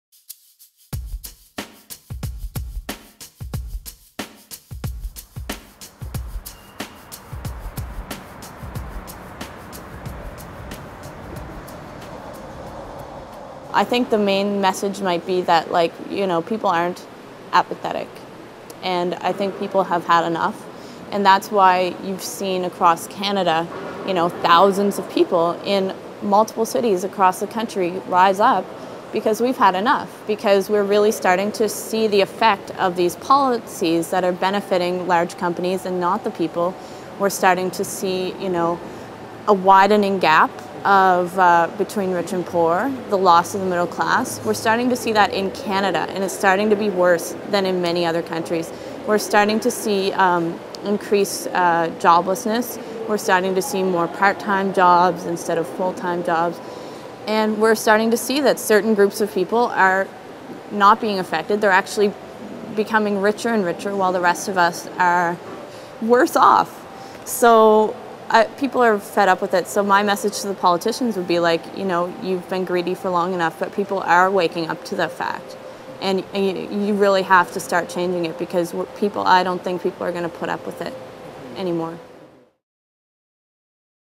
Occupy Vancouver: A message to BC Politicians
"If you could address all the members of B.C.'s legislature, what would be your message?"

Occupy Vancouver participants in conversation with IntegrityBC.

IntegrityBC's effort is to help restore a bond built on trust and confidence between citizens and their elected officials.